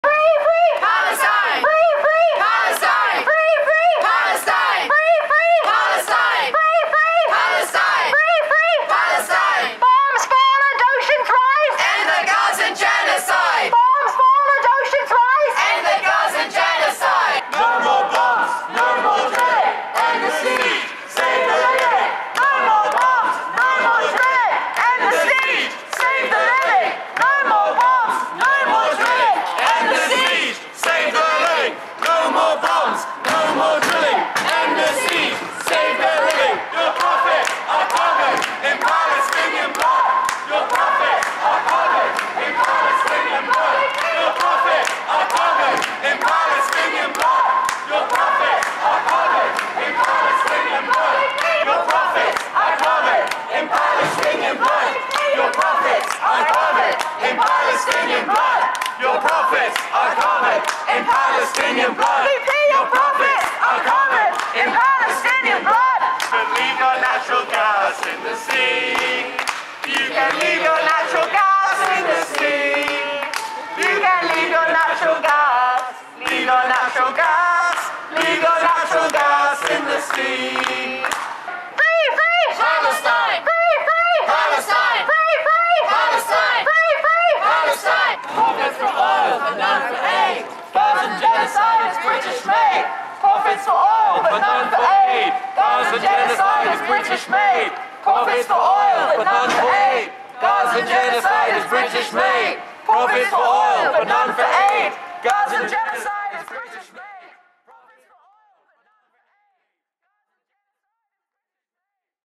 Palestine: Fossil Free London blockade BP HQ for profiting from genocide
Fossil Free London shut down BP's headquarters in London today - for profitting from Israel's genocide in Gaza.

In the last few weeks, amidst genocide, Israel has given out 12 gas exploration licences off the coast of Gaza to 6 companies, including BP plc.

Not only have BP received this licence, they’re also on track to acquire 50% of Delek Group-owned NewMed, who have also been granted a licence.

In addition - Infosys, in May, signed a £1.5billion deal with BP. Rishi Sunak’s wife has major shares in Infosys.

Israel, BP, Sunak, all ripping profit from the genocide of Palestine and exploitation of the people and land - and this is on top of Israel's continuing role in the Middle East in protecting Western capitalism's oil interests in the region.

The lesson is clear: if we are to move away from fossil fuels to renewables in a global just transition that benefits workers and communities, a Free Palestine is absolutely essential. And if the rapidly increasing global movement for a Free Palestine manages to stop Israel's horrific genocide, we coud be looking at a movement powerful enough to change everything that's wrong in this world.

To find out more, and to join the campaign to stop gas exploration off the coast of Gaza, go to fossilfreelondon.org.